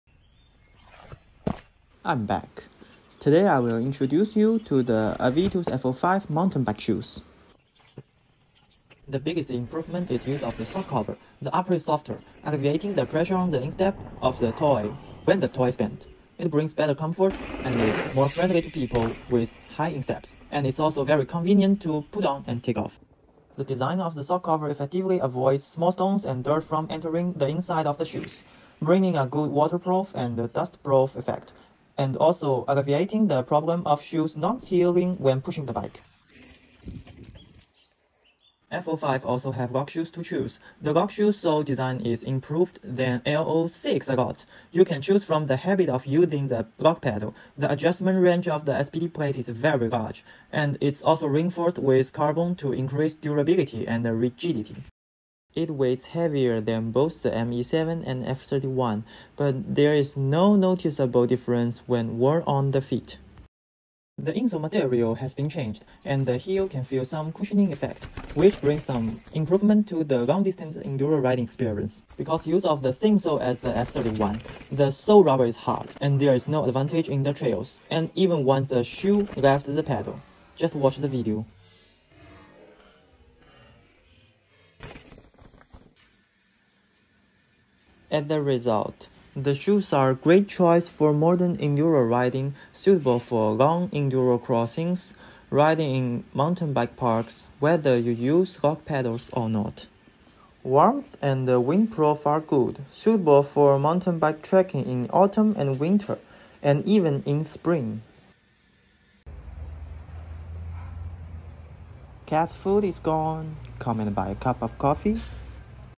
MTB Shoes Introduction: Avitus F05 MTB Shoes
Evaluate Avitus F05 MTB shoes in Hangzhou Jiulisong in China.